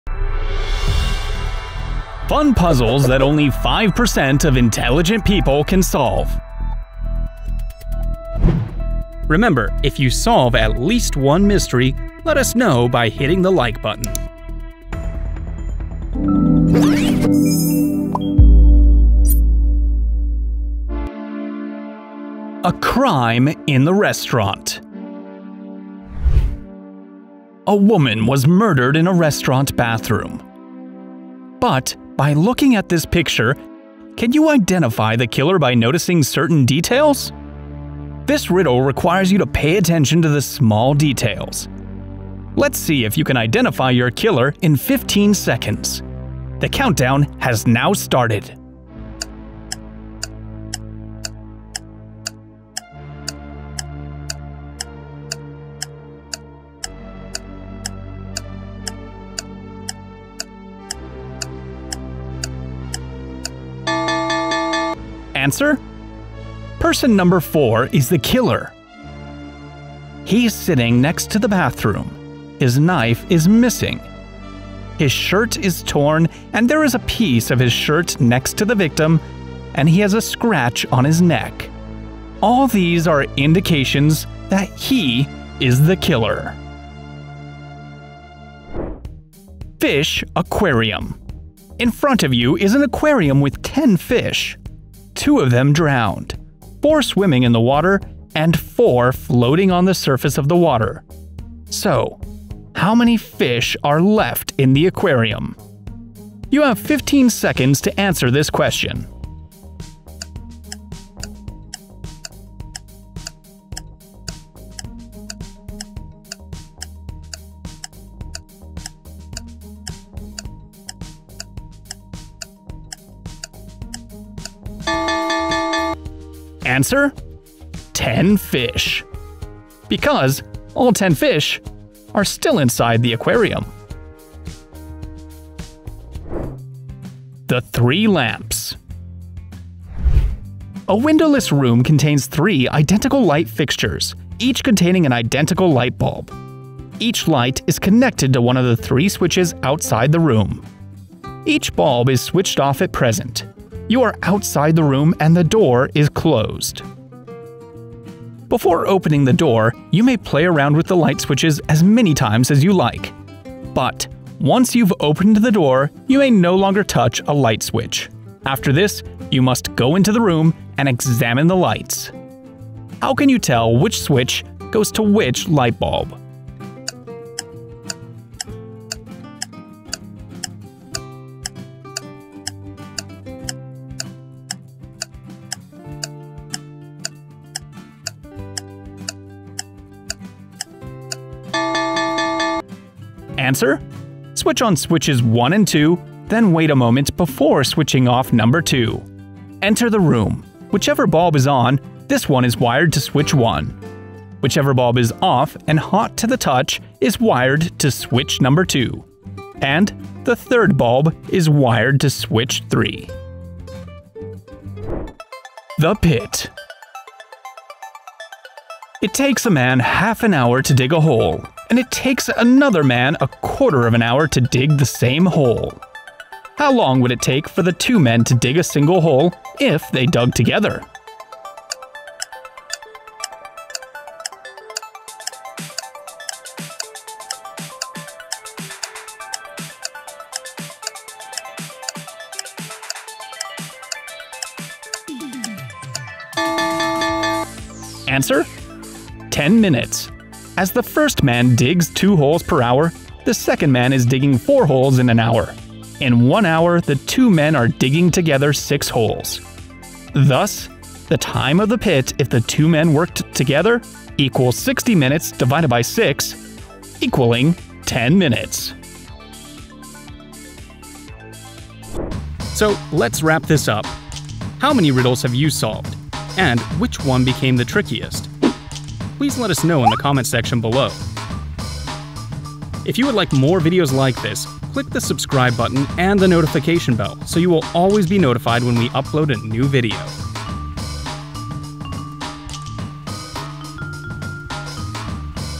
Who is the killer? | New Riddles and Brain Teasers with Answers
New riddles and brain teasers with answers with voice-only smart people can solve.
Are you ready to expand your mind? I'm sure all of you can solve these tricky puzzles.
All you need is to start watching this video and pay attention to every detail.

⏱️Timestamps⏱️
00:00 Start with these new riddles and brain teasers with answers
00:22 A crime in the restaurant | Speed up your thinking with this Riddle
01:29 Fish Aquarium  | Riddle for genius people
02:16 The Three Lamps | This difficult riddle will increase your IQ
03:35 The pit | An easy puzzle to test your smarts

✔️ SUMMARY✔️
► Riddle#1 | A crime in the restaurant - Who is the killer ?
A woman was murdered in a restaurant bathroom, but by looking at this picture, can you identify the killer by noticing certain details?
This riddle requires you to pay attention to the small details.

► Riddle#2 | Fish Aquarium
In front of you is an aquarium with 10 fish. Two of them drowned, four swimming in the water, and four floating on the surface of the water, so how many fish are left in the aquarium.

► Riddle #3 | The Three Lamps
A windowless room contains three identical light fixtures, each containing an identical light bulb. Each light is connected to one of three switches outside the room.

► Riddle #4 | The pit
It takes a man half an hour to dig a hole.
And it takes another man a quarter of an hour to dig the same hole
How long would it take for the two men to dig a single hole if they dug together?